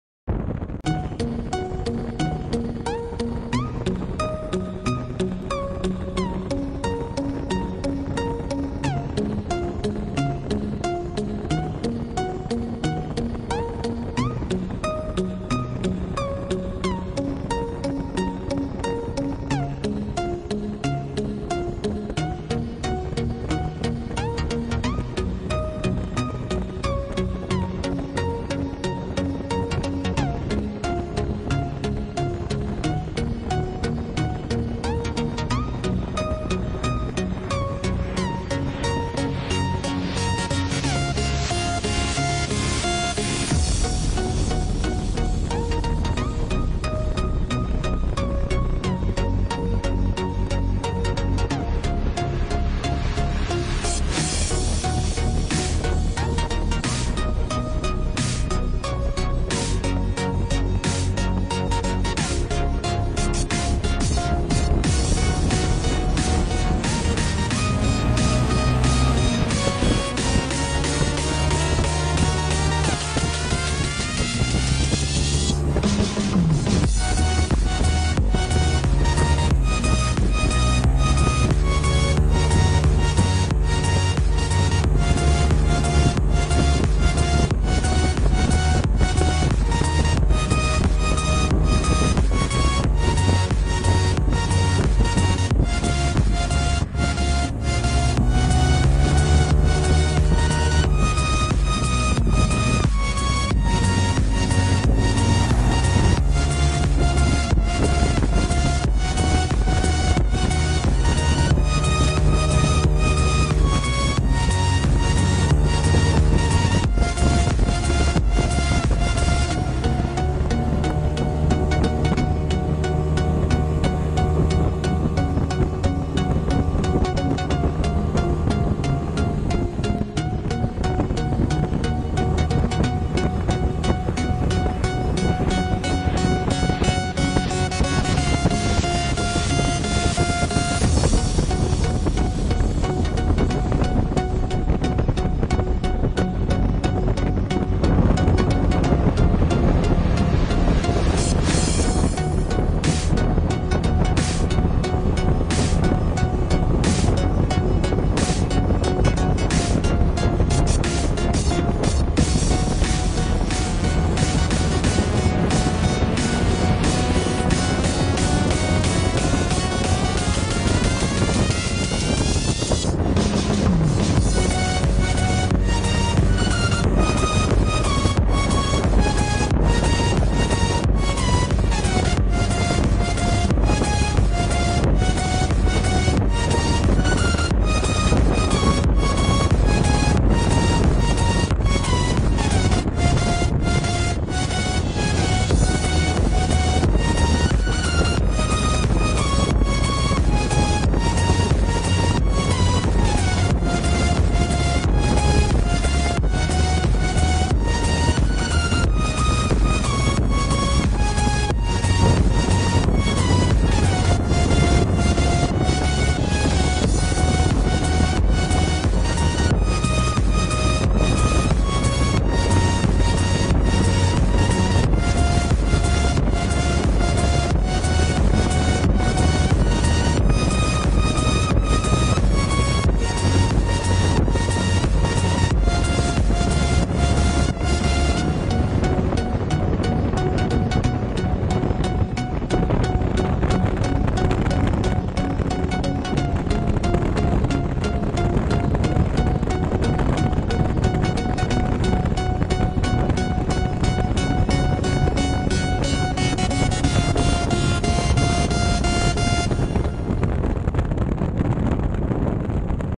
Yamaha Xmax - Ruta Frio y Niebla
Ruta hacia la concentracion de Los HUevos Fritos en Sacedon con Yamaha XMAX y Suzuki VSTROM